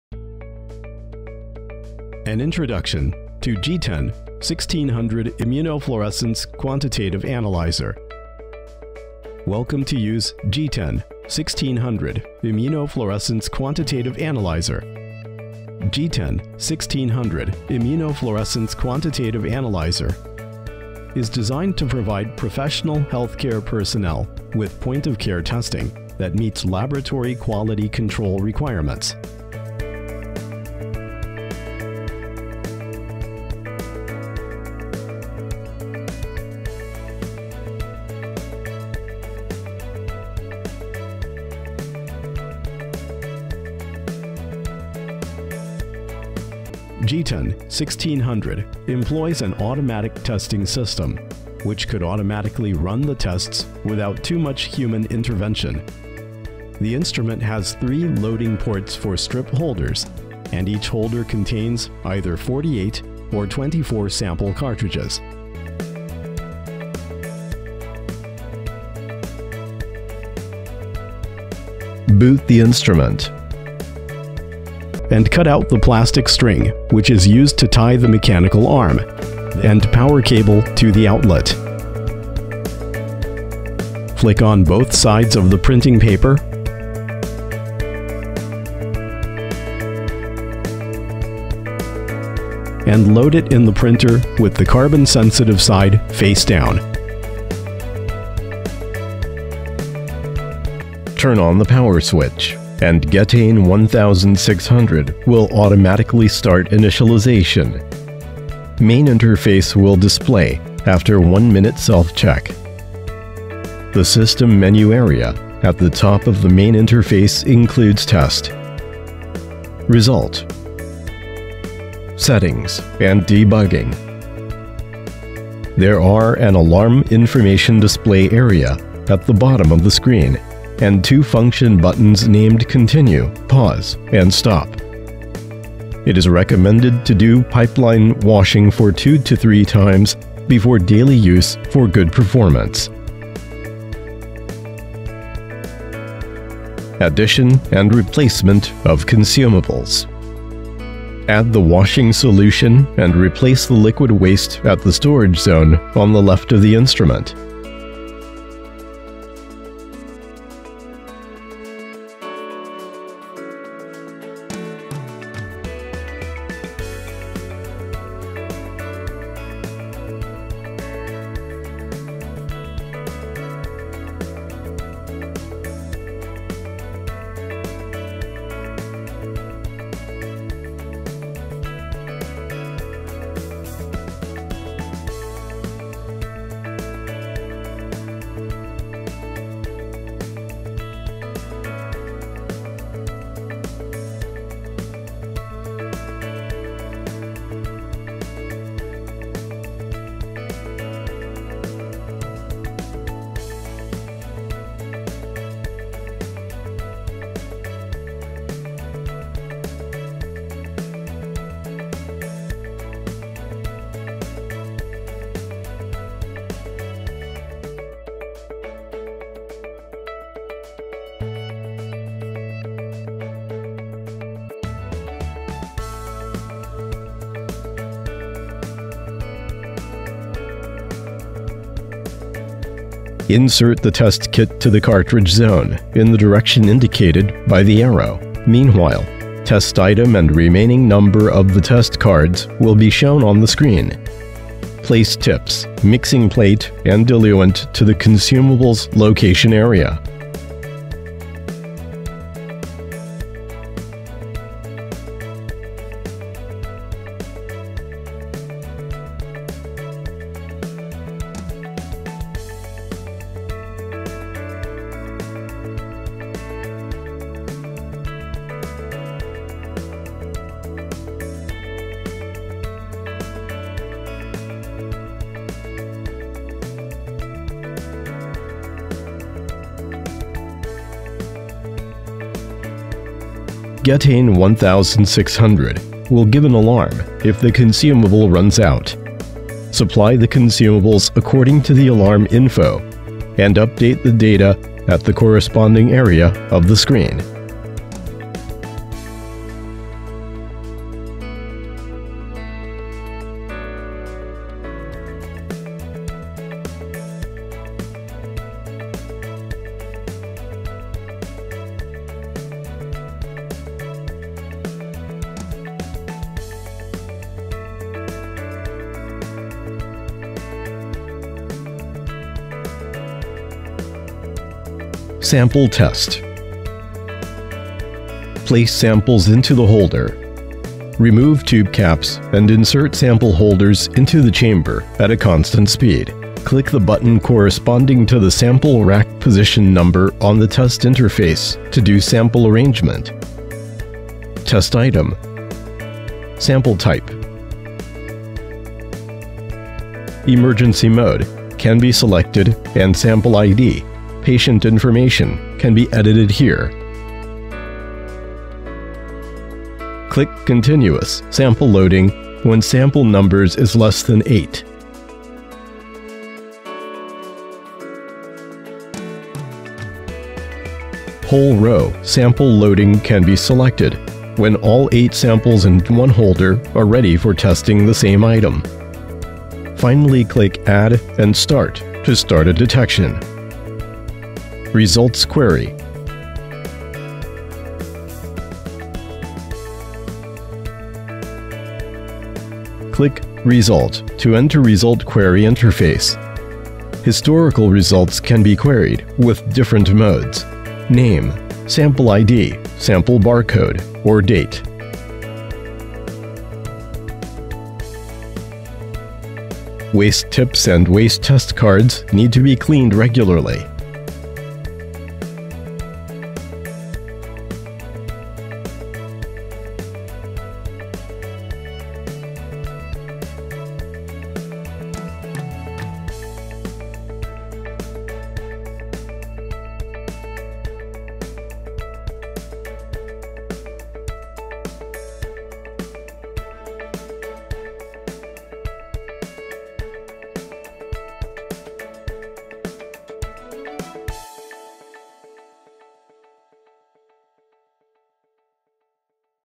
Getein 1600 Immunofluorescence Analyzer Operation Guide (POCT)
Getein 1600 Immunofluorescence Analyzer
In conjunction with dedicated test kits for Immunofluorescence, the instrument automates the detection and quantification of markers for cardiovascular disease, renal diseases, inflammation, fertility, diabetes mellitus, bone metabolism, tumor and thyroid in a biological sample.